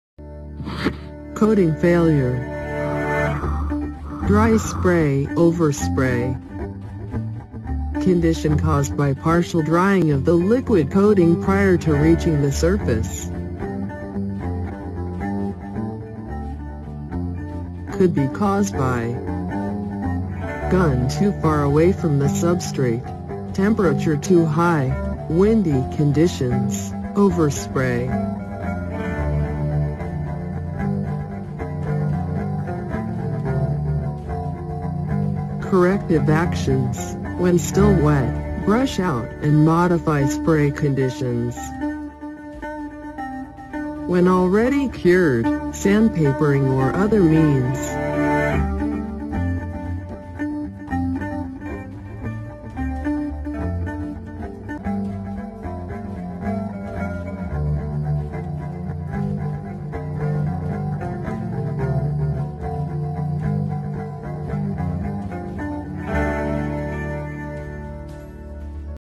Coating failure ( dry spray and over spray)##
Dear friends, if you are working in paint shop then you might have faced dry spray and over spray kind of paint problem on the components. So, in this session will learn how we can solve and over-come throughout these kind of problems.